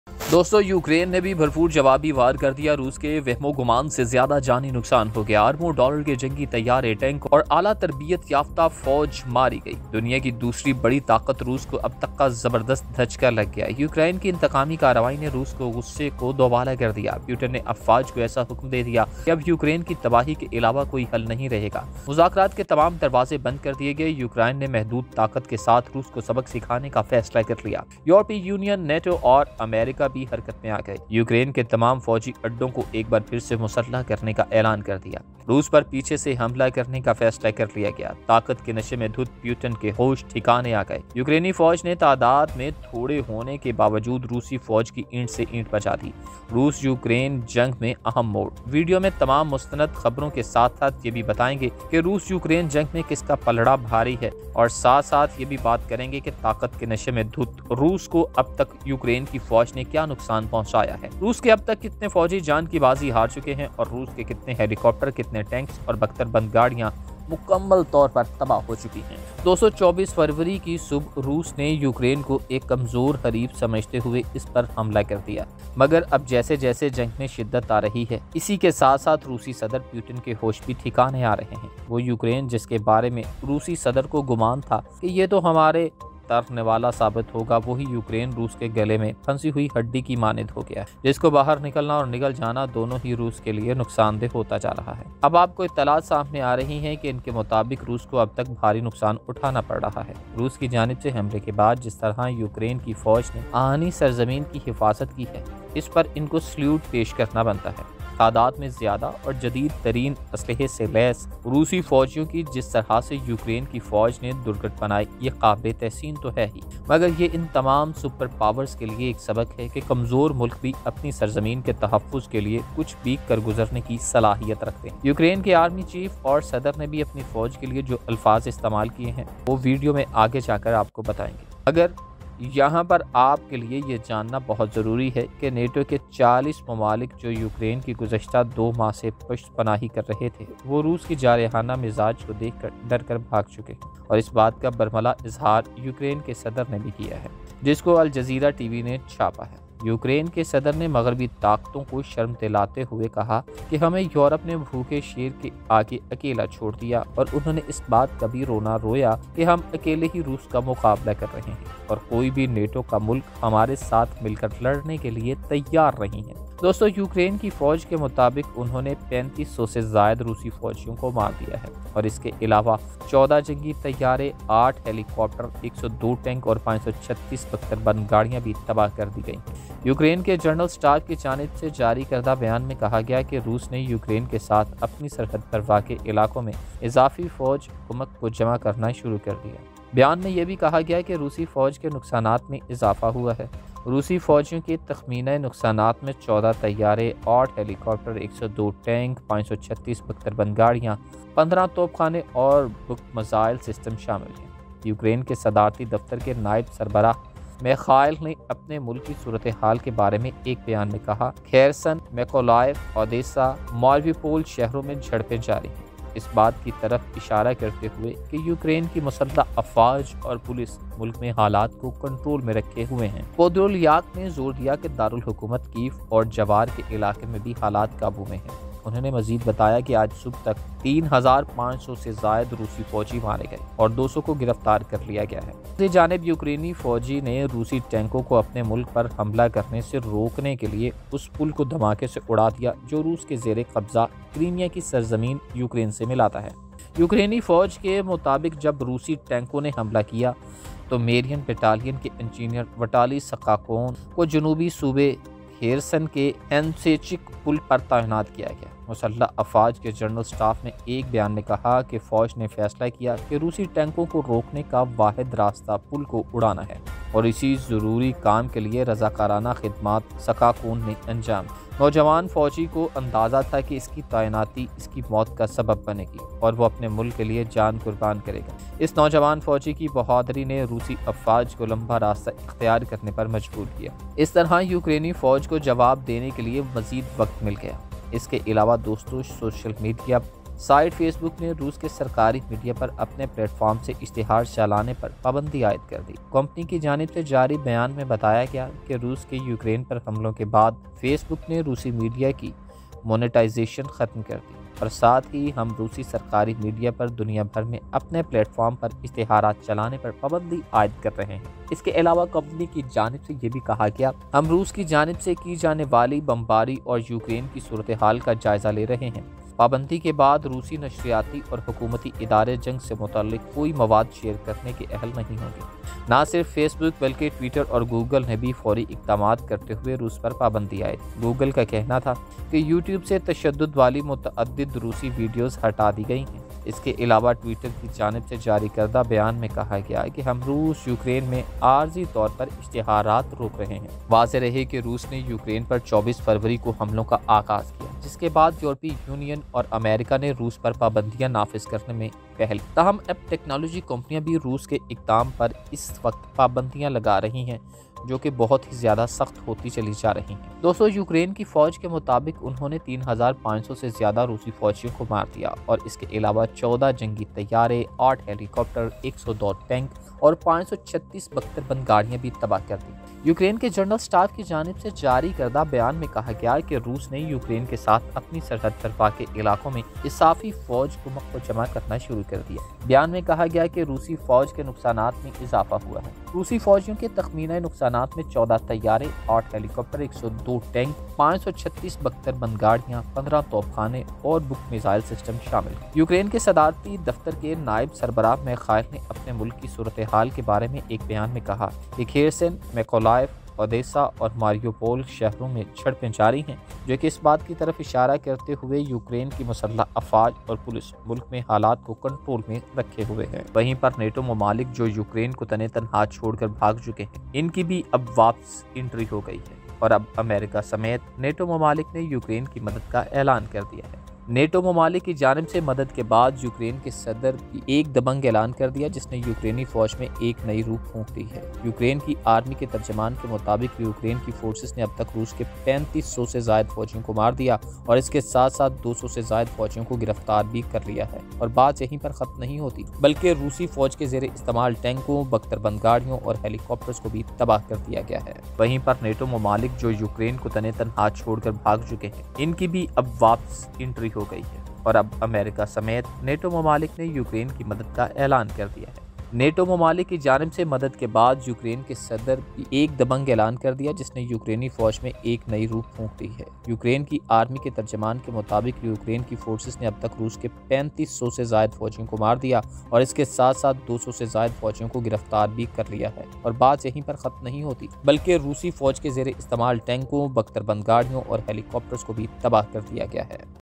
Putin Angry On Ukraine As 5000 Russian Troops Neutralized By Ukrainian Army | Kyiv
Urdu
Hindi
English
Subtitles